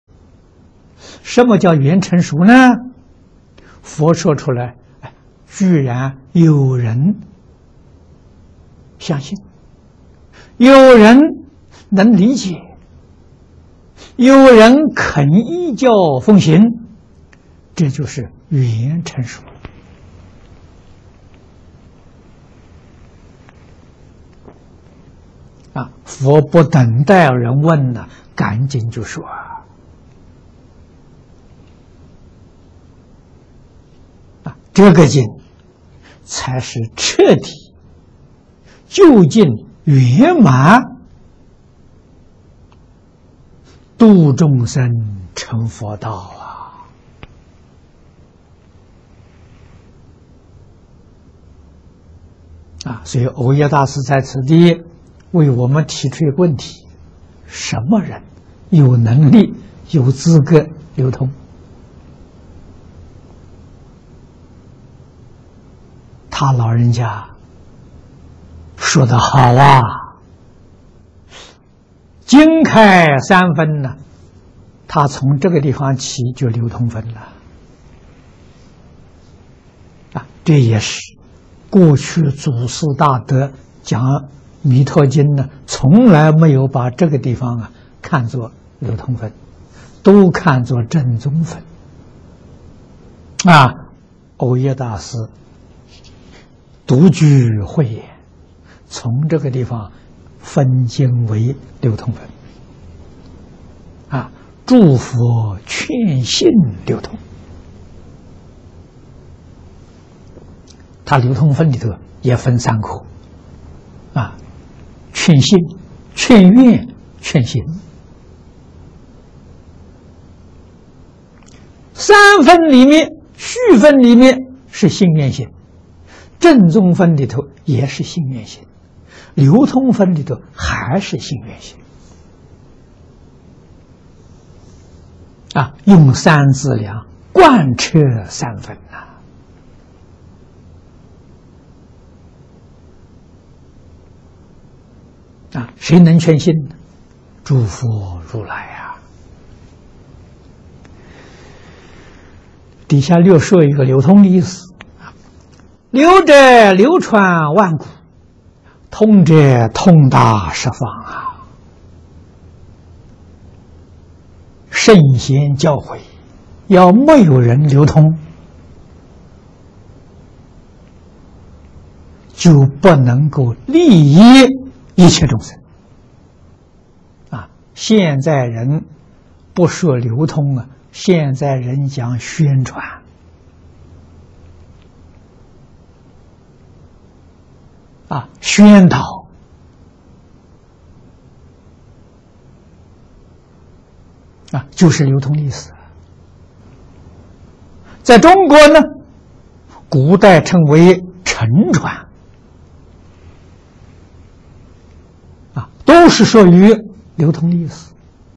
淨空老法師:中峰三時繫念法事全集（第五十七集）03/16
節錄自~檔名：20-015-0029，澳洲淨宗學院

影片字幕：
什麼叫緣成熟？佛說出來，居然有人相信，有人能理解，有人肯依教奉行，這就是緣成熟了。佛不等待人問，趕緊就說。這個經才是徹底、究竟圓滿度眾生成佛道！
       蕅益大師在此地為我們提出一個問題：什麼人有能力、有資格流通？他老人家說得好！經開三分，他從這個地方起就分為流通分。這也是過去祖師大德講《彌陀經》，從來沒有把這個地方看作流通分，都看作正宗分。蕅益大師獨具慧眼，從這個地方分經為流通分，「諸佛勸信流通」。他流通分裡頭也分三科，勸信、勸願、勸行。三分裡面，序分裡面是信願行，正宗分裡頭也是信願行，流通分裡頭還是信願行，用三資糧貫徹三分。誰能勸信？諸佛如來！
       底下略說流通的意思，「流者，流傳萬古，通者，通達十方」，聖賢教誨要沒有人流通，就不能夠利益一切眾生。現在人不說流通，現在人講宣傳、宣導，就是流通的意思；在中國古代稱為承傳，都是屬於流通的意思。